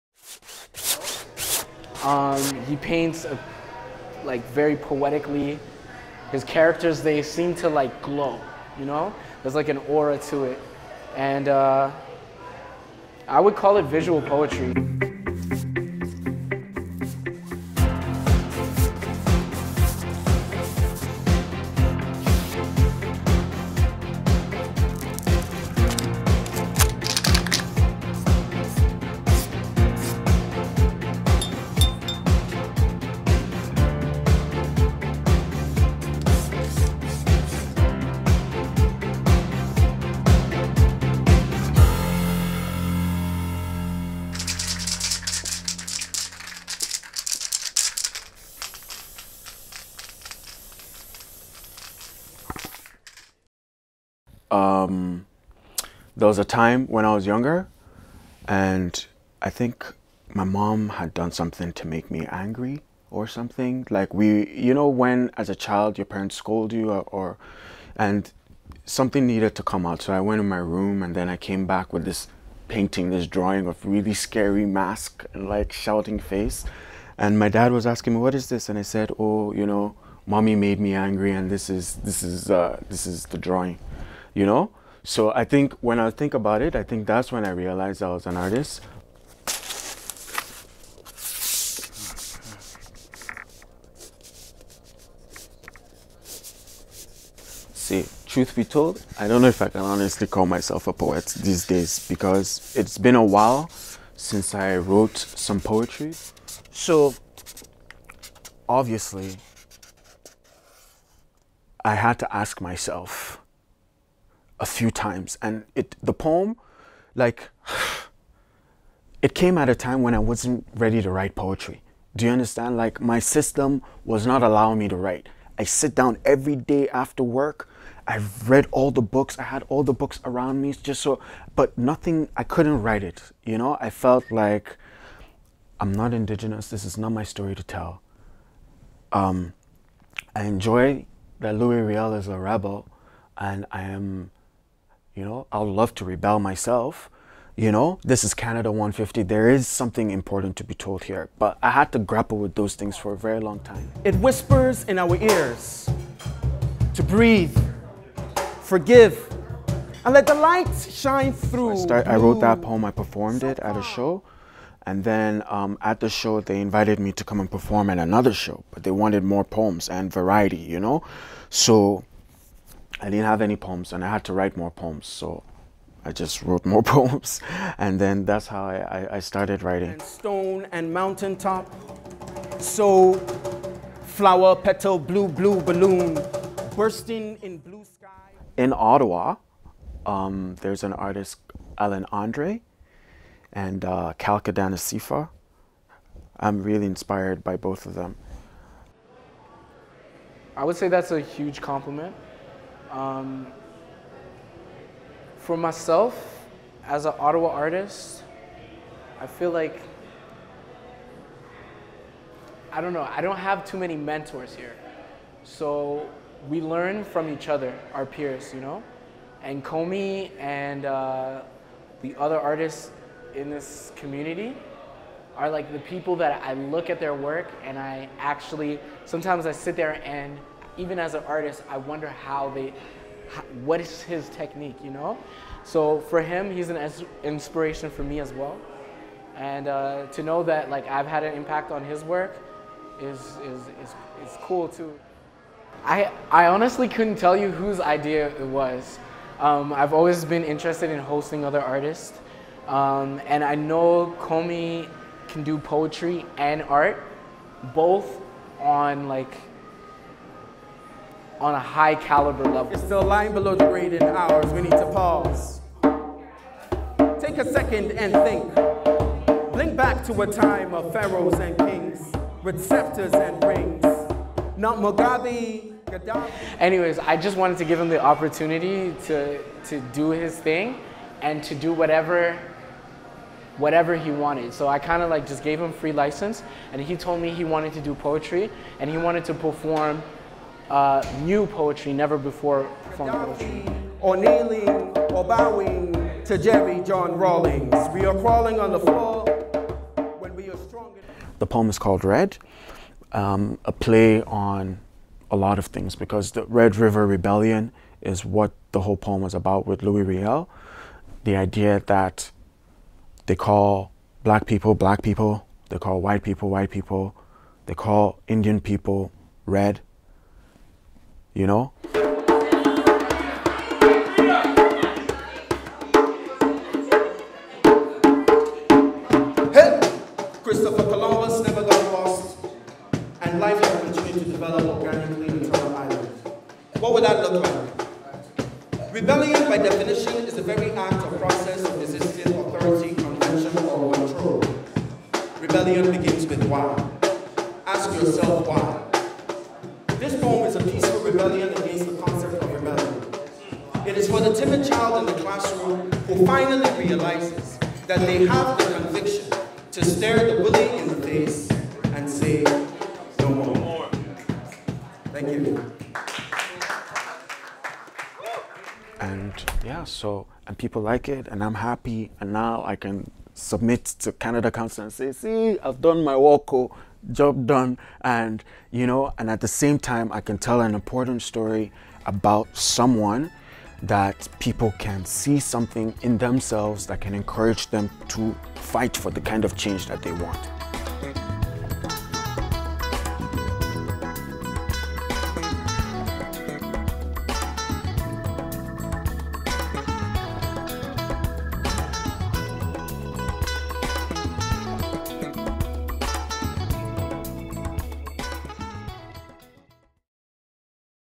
Red River
A Nigerian artist decides to write a poem addressing a chapter of Canadian history.

"It takes a lot of courage to tackle a subject such as this."

SPECIAL THANKS TO:
Komi Olaf
Alan Andre
Grounded Coffee Lounge, Ottawa